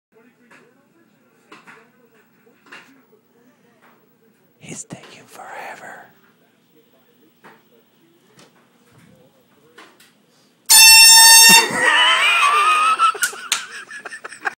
Air Horn shock!!
Scaring the shit out of Eric!!